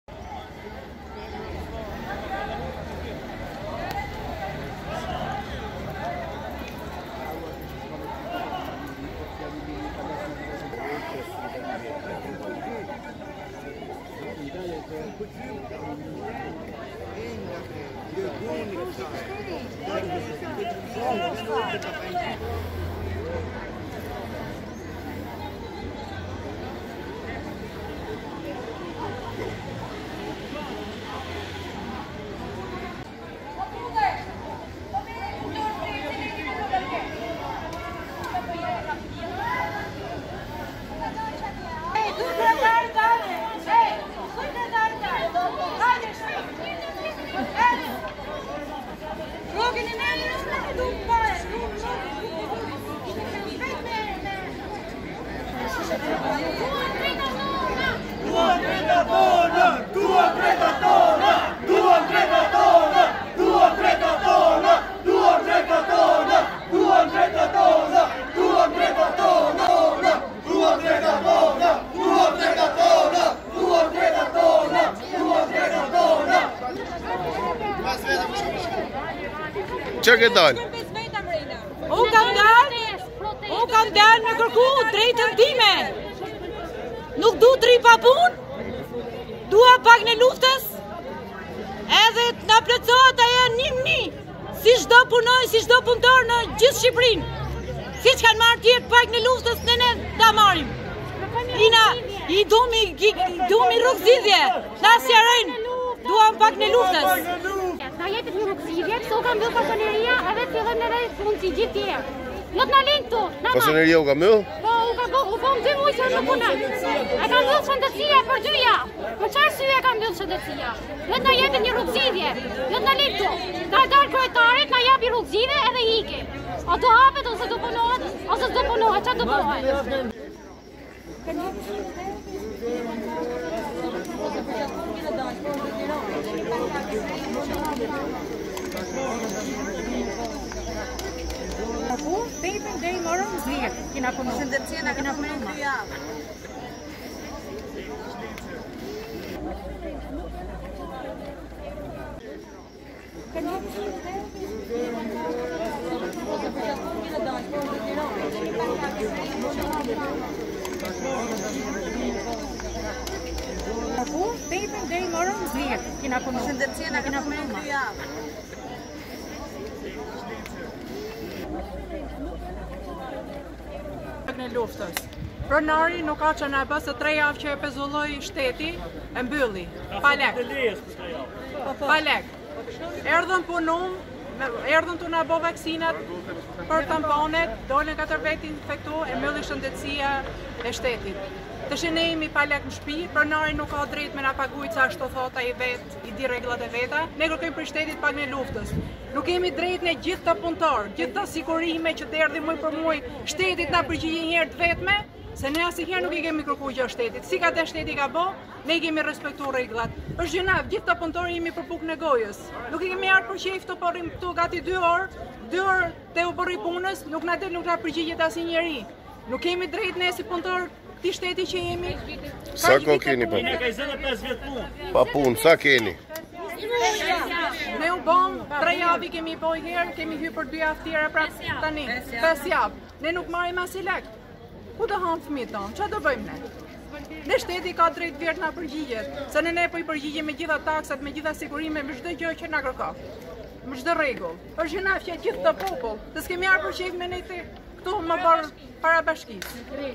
400 punëtorë të një fasonerie në Krujë protestë para bashkisë për pagën e luftës
Rreth 400 punëtorë të fasonerisë 'Altek shpk' janë ngritur në protestë përpara bashkisë Krujë ditën e sotme. Punonjësit e kësaj fasonerie kërkojnë nga qeveria pagën e luftës, që sipas pretendimit të tyre i takon.

Gjatë gjithë kohës së pandemisë kemi punuar me ditë thonë ata dhe nuk kemi përfituar as rrogë nga firma.

"Të na jepet një rrugëzgjidhje, se është mbyllur fasoneria dhe ne duhet të punojmë si të gjithë të tjerët. Kam dy muaj që nuk jam në punë, e ka mbyllur Ministria e Shëndetësisë. Të dalë kryetari dhe të na japë një rrugëzgjidhje, ta dimë nëse do hapet apo jo,"-shprehet një protestuese.

"Duam pagën e luftës, sepse pronari nuk ka çfarë bën pasi për tre javë që e pezulloi shteti e mbylli. Erdhëm për të punuar dhe na bënë tamponët ku katër veta dolën pozitivë. Më pas e mbylli ministria. Tani ne jemi pa lekë në shtëpi dhe pronari nuk na paguan se thotë ashtu është ligji. Ne kërkojmë nga shteti pagën e luftës.

A nuk kemi të drejtë ne, gjithë këto puntorë, gjithë këto sigurime që derdhim çdo muaj, shteti të na përgjigjet njëherë të vetme? Ne asnjëherë si kemi kërkuar gjë shtetit. Kemi 25 vjet që punojmë,"-shprehet një tjetër puntore.

Gjatë protestës punonjësit mbajnë edhe pankarta në duar ku lexohet: "Ku do hanë fëmijët tanë?" "Do mbushim rrugët dhe sheshet." "Po luani ping pong me ne!!" "Kërkojmë pagën e luftës".